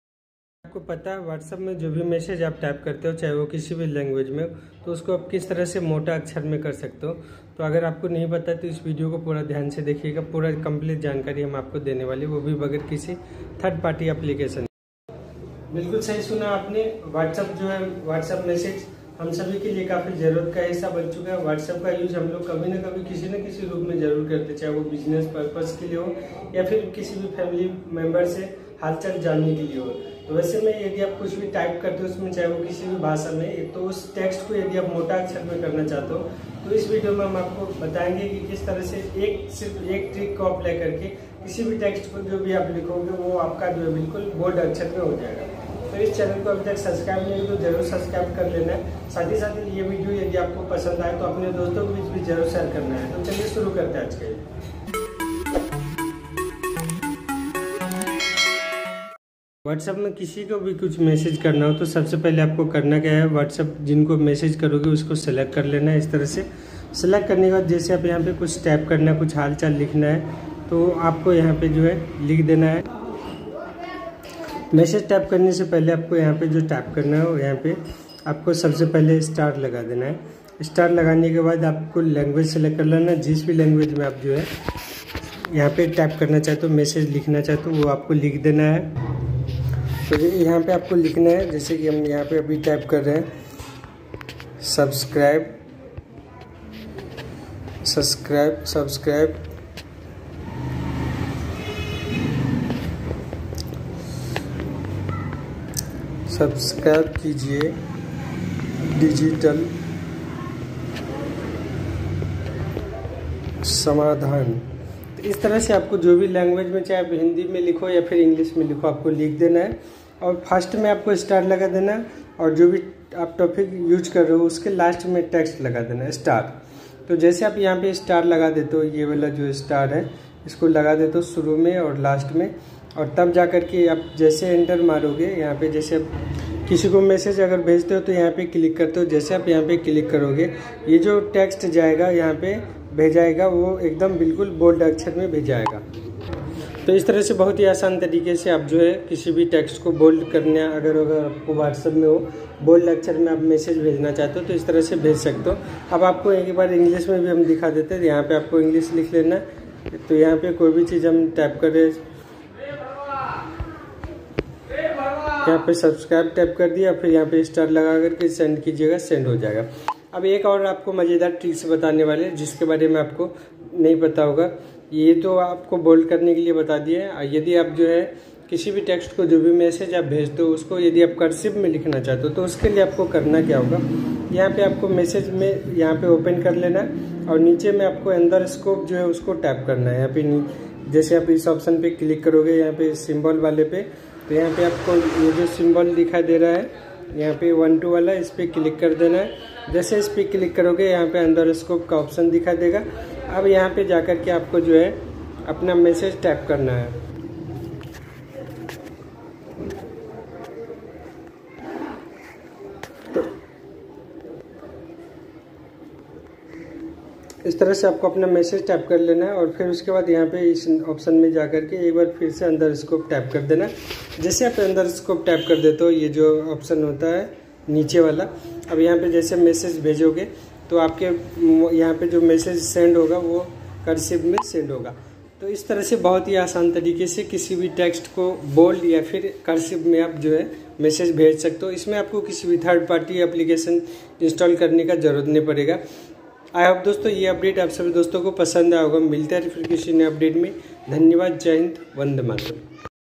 व्हाट्सएप्प मे मोटा अक्षर मे कैसे लिखे।how to write bold text on whatsapp🙏❤️
✍️इस वीडियो मे btaye है कि व्हाट्सएप्प me मोटा अक्षर मे कैसे लिखे